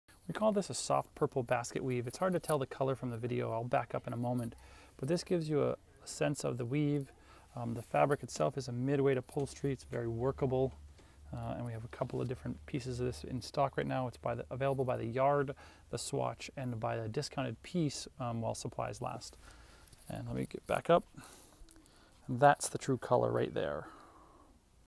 Soft Purple Basket Weave 54" Wide Upholstery Fabric
Sold by the yard this heavy weight upholstery fabric has a basket
weave appearance in a gorgeous shade of lavender. With an incredibly
soft feel this fabric would be perfect for any kind of couch or chair
upholstery project, cushions, floor poufs. This would also be a great
fabric for bag making.

54" Wide.

Purple in color.

Mid to heavy weight.

Upholstery, pillows or home decor projects.

Sold by the yard and by the swatch.